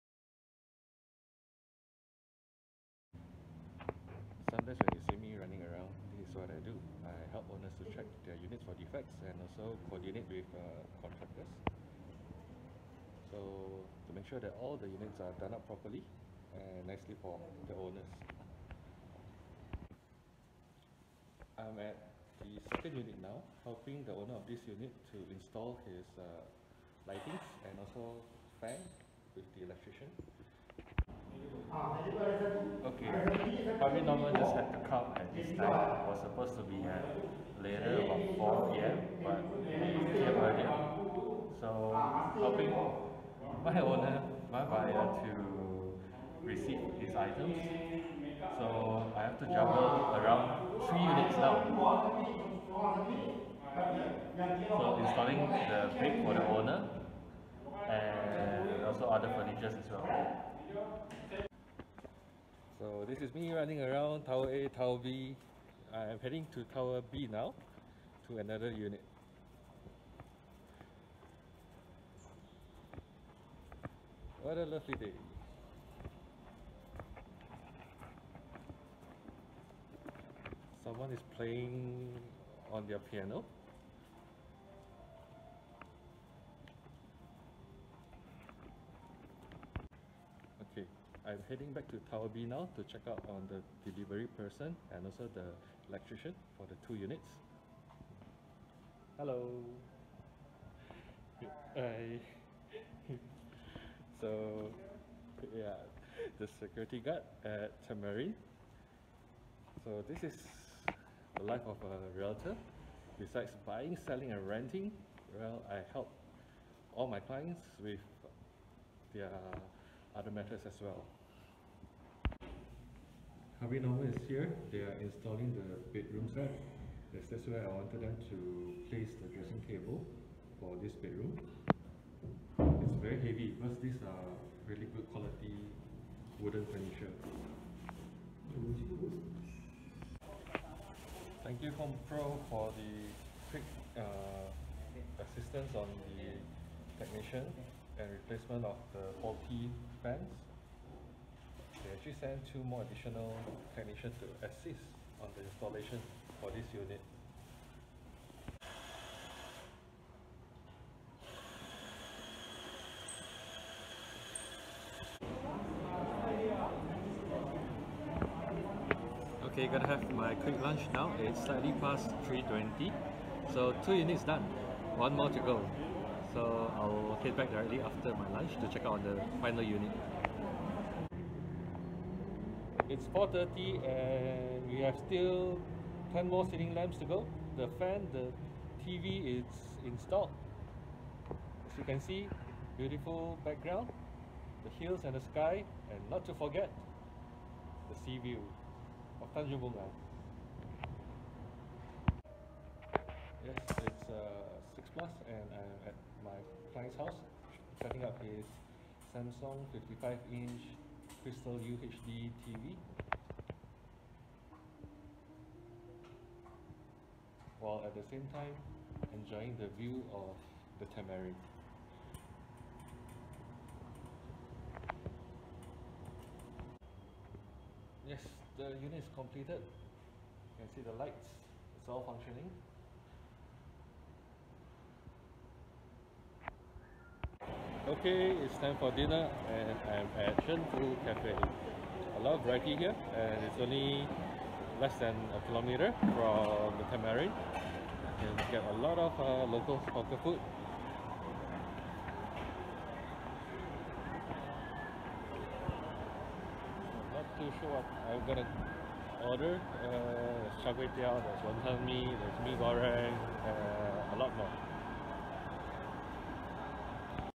What I do besides Buying, Selling and Renting | Vincent Lee Vlog #1
Being a realtor is not just about Buying, Selling and Renting.

At times, I clear a day from appointment because I will help my client with their other matters.

Such as Checking for Defects in their unit, assisting with Electrical Items installation, receiving their goods such as furniture, installing of curtains and blinds, arranging cleaning of the property, receiving parcels, paying bills and other matters.

At the end of the day, I ask myself. How to Make Money by Working for Free?